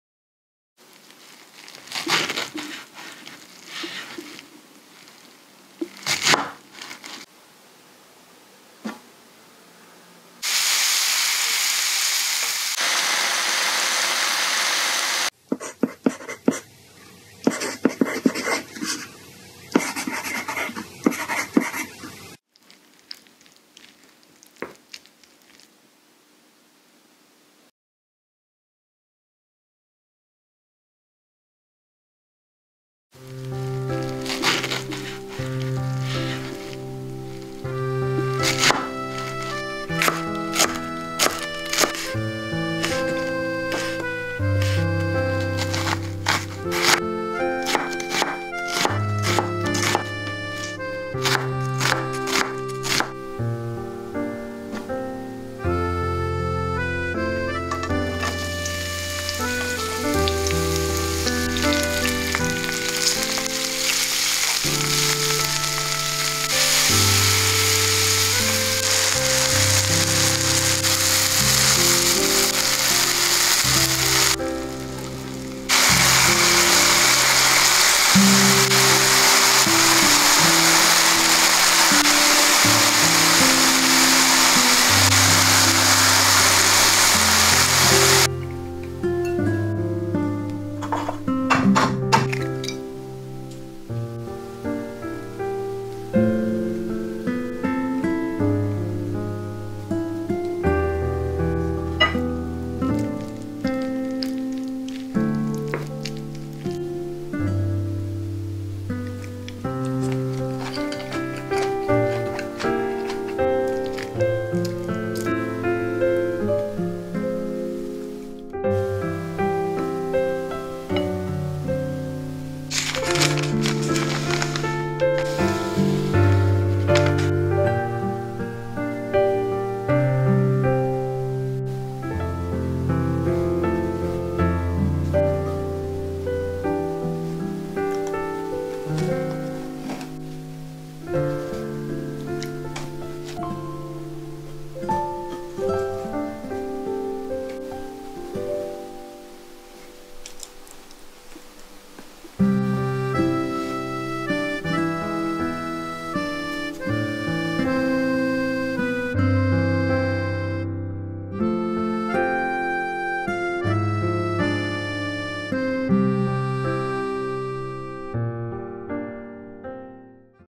500원으로 만드는 중화식 팽이버섯 덮밥 Rice with enoki mushrooms
단돈 500원 팽이벗섯으로 만든 중화식 팽이버섯 덮밥
노른자를 톡 터트려서 먹으면 더욱 맛있어요:)

* 자세한 레시피는 네이버 블로거를 참고해주세요
* Please refer to Naver Blogger for the detailed recipe.

[재료]
- 팽이버섯 150g
- 대파 1/2대
- 양파 1개
- 진간장 2T
- 맛술 2T
- 참깨 1/2T
- 참기름 1/2T
- 달걀 1알

[Ingredient]
- Enoki mushroom 150g
- Green onion 1/2
- Onion 1
- Thick soy sauce 2T
- Cooking wine 2T
- Sesame seeds 1/2T
- Sesame oil 1/2T
- Egg 1